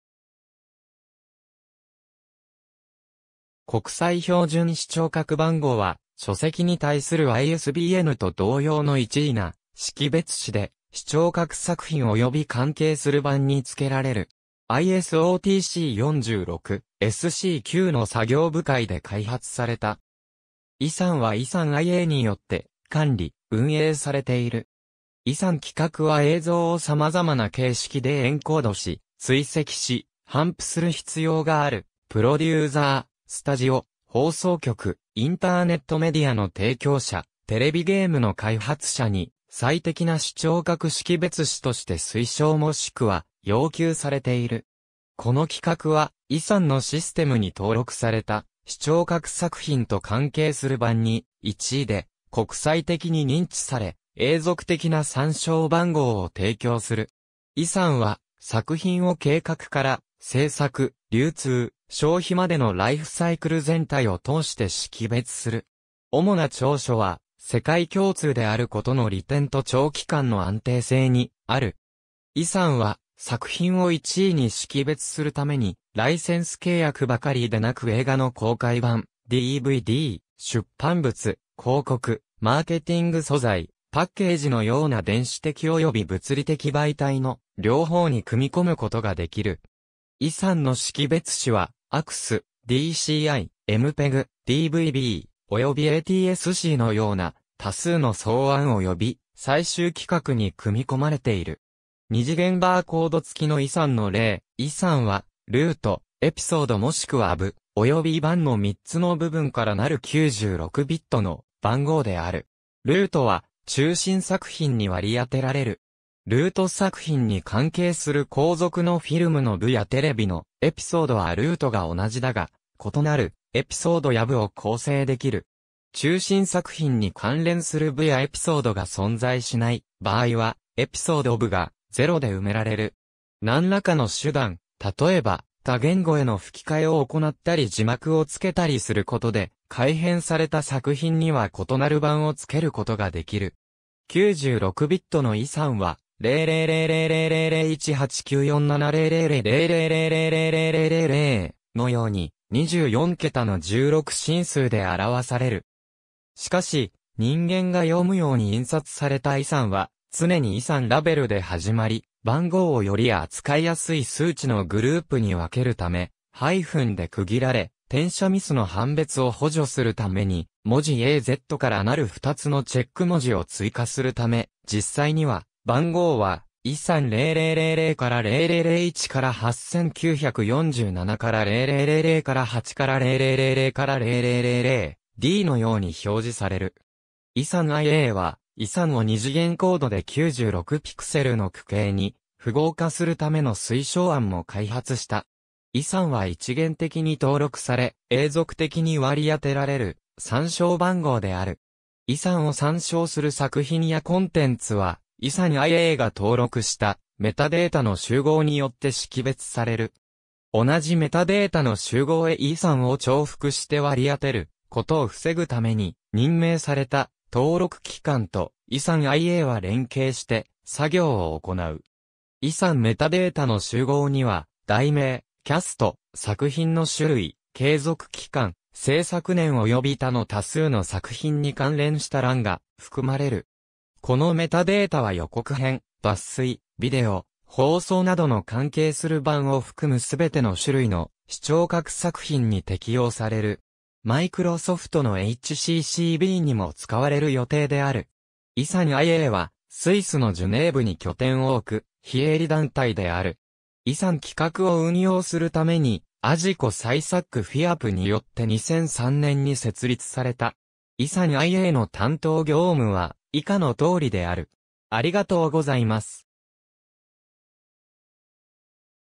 国際標準視聴覚番号
国際標準視聴覚番号（こくさいひょうじゅんしちょうかくばんごう、International Standard Audiovisual Number, ISAN）は、書籍に対するISBNと同様の一意な識別子で、視聴覚作品および関係する版に付けられる。
ISO (国際標準化機構) TC46/SC9の作業部会で開発された。
ISANはISAN-IAによって管理・運営されている。
ISAN規格（ISO規格 15706:2002およびISO 15706-2）は映像をさまざまな形式でエンコードし、追跡し、頒布する必要があるプロデューザー、スタジオ、放送局、インターネットメディアの提供者、テレビゲームの開発者に最適な視聴覚識別子として推奨もしくは要求されている。
この規格はISANのシステムに登録された視聴覚作品と関係する版に、一意で、国際的に認知され、永続的な参照番号を提供する。
ISANは作品を計画から制作、流通、消費までのライフサイクル全体を通して識別する。
おもな長所は世界共通であることの利点と長期間の安定性にある。
ISANは作品を一意に識別するために、ライセンス契約ばかりでなく映画の公開版、DVD、出版物、広告、マーケティング素材、パッケージのような電子的および物理的媒体の両方に組み込むことができる。
ISANの識別子はAACS, DCI, MPEG, DVB, およびATSCのような、多数の草案および最終規格に組み込まれている。
二次元バーコード付きのISANの例 ISANはルート、エピソードもしくは部、および版の3つの部分からなる96ビットの番号である。
ルートは中心作品に割り当てられる。
ルート作品に関係する後続のフィルムの部やテレビのエピソードはルートが同じだが、異なる「エピソードや部」を構成できる。
中心作品に関連する部やエピソードが存在しない場合は、エピソード部がゼロで埋められる。
何らかの手段、たとえば、他言語への吹き替えを行ったり字幕を付けたりすることで改変された作品（およびそのエピソードや部）には異なる版を付けることができる。
96ビットのISANは「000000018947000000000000」のように、24桁の16進数であらわされる。
しかし、人間が読むように印刷されたISANは常にISANラベルで始まり、番号をより扱いやすい数値のグループに分けるためハイフンで区切られ、転写ミスの判別を補助するために文字A–Zからなる2つのチェック文字を追加するため、実際には番号は「ISAN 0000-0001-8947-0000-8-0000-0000-D」のように表示される。
ISAN-IAはISANを二次元コードで96ピクセルの矩形に符号化するための推奨案も開発した。
ISANは一元的に登録され、永続的に割り当てられる参照番号である。
ISANを参照する作品やコンテンツはISAN-IAが登録したメタデータの集合によって識別される。
同じメタデータの集合へISANを重複して割り当てることを防ぐために、任命された登録機関とISAN-IAは連携して作業を行う。
ISANメタデータの集合には題名（原題と副題）、キャスト（監督、俳優、プロデューサー、脚本家など）、作品の種類（映画、ドキュメンタリー、テレビシリーズや娯楽作品、スポーツ大会、テレビゲームなど）、継続期間、制作年および他の多数の作品に関連した欄が含まれる。
このメタデータは予告編、抜粋、ビデオ、放送などの関係する版を含むすべての種類の視聴覚作品に適用される。
マイクロソフトのHCCB (High Capacity Color Barcode) にも使われる予定である。
ISAN-IA（ISAN International Agency, 国際標準視聴覚資料番号機構）はスイスのジュネーヴに拠点を置く非営利団体である。
ISAN規格を運用するために、AGICOA・CISAC・FIAPFによって2003年に設立された。
ISAN-IAの担当業務は以下のとおりである。